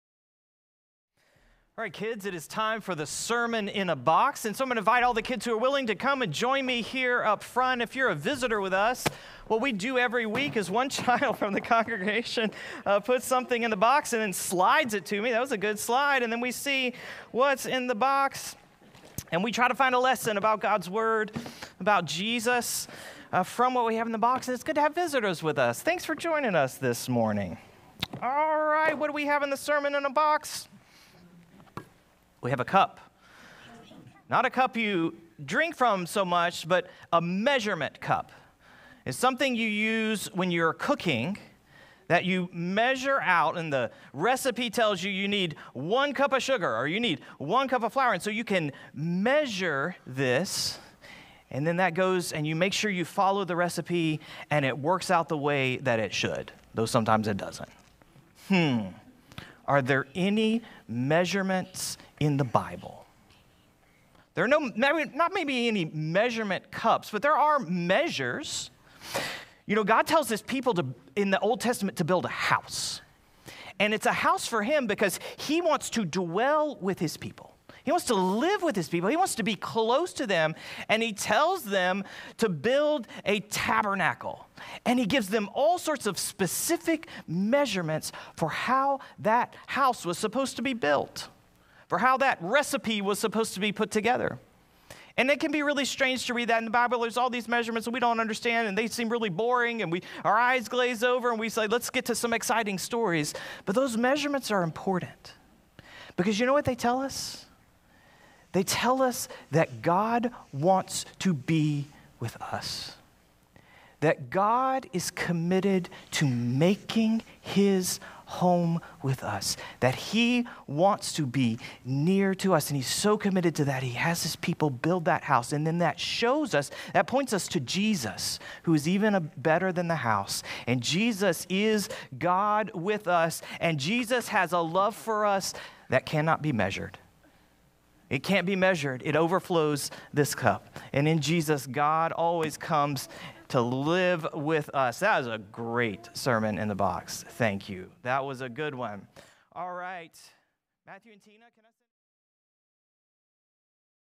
Sermon in a Box October 23, 2022
Each Sunday, we give a box out to one of the children and have them try to stump the Pastor on the next Sunday. Today, Pastor Jonathon delivered an object lesson on a measuring cup.